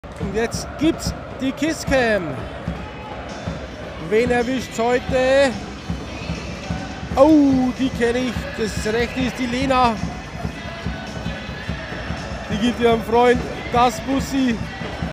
Kiss Cam vom 24.02.2023 - Eisbären Regensburg - Lausitzer Füchse
Die Kiss Cam beim Heimspiel gegen die Lausitzer Füchse